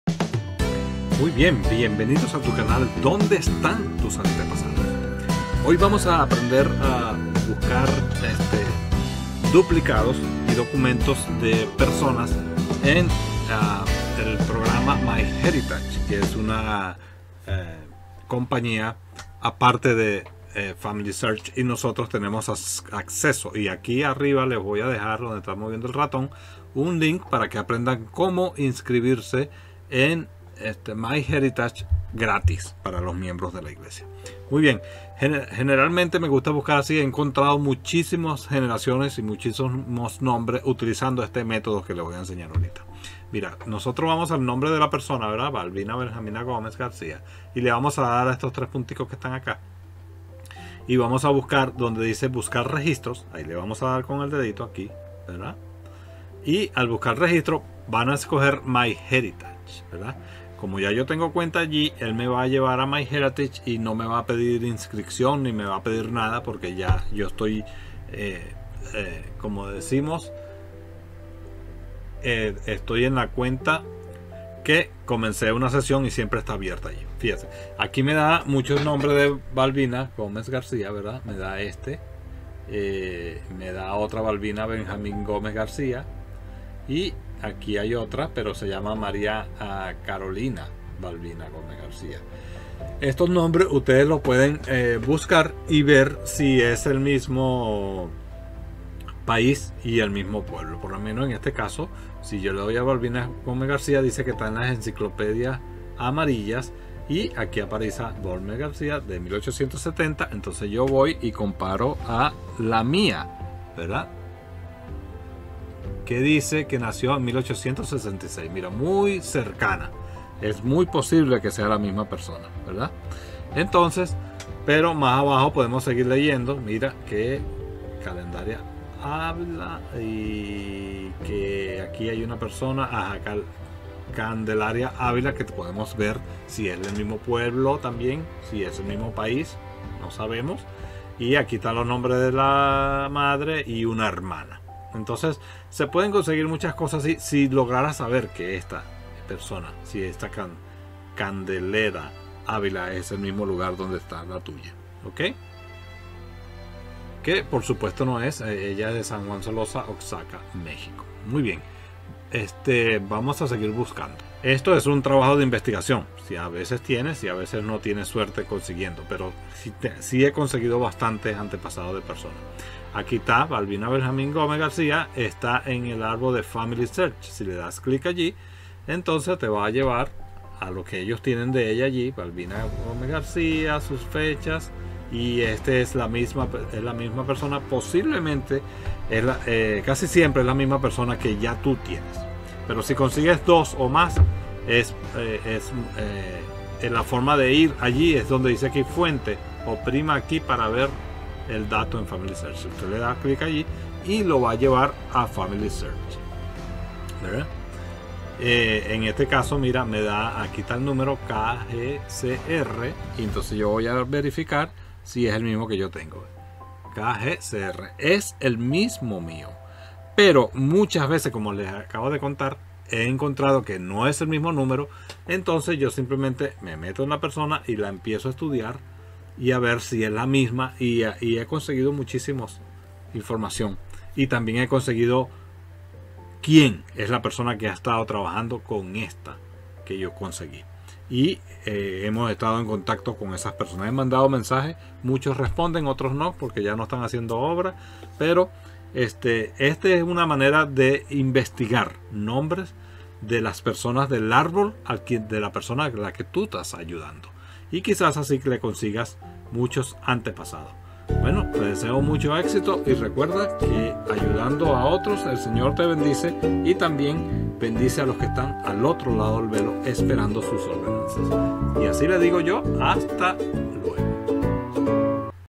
Buscar antepasados en MyHeritage
Aprender a buscar antepasados y documentos en MyHeritage

Visita a la escuela de familySearch y ve todos Las lecciones para que aprendas hacer tu historia familiar y ayudar a otros: